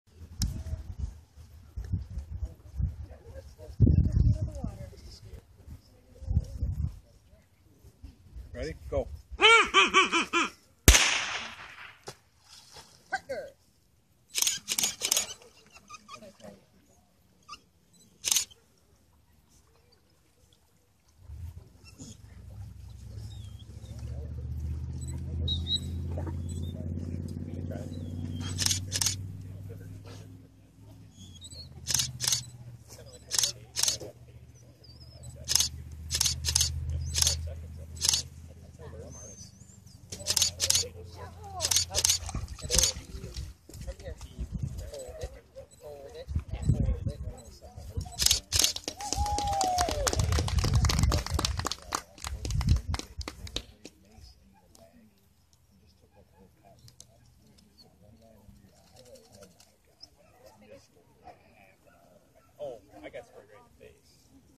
Field Spaniel Partner Junior Hunt Test Water Retrieve Calicos Partner in Crime
Field Spaniel, Partner, at his first attempt at a Hunt Test and this is showing how he did on the water retrieve exercise. This is the final exercise that they completed that gave Partner his first qualification towards his Junior Hunter Title.
Fox River Field Spaniel Club Hunt Test on October 20, 2012 with Judges Dawn Schuster and Rebecca Kruger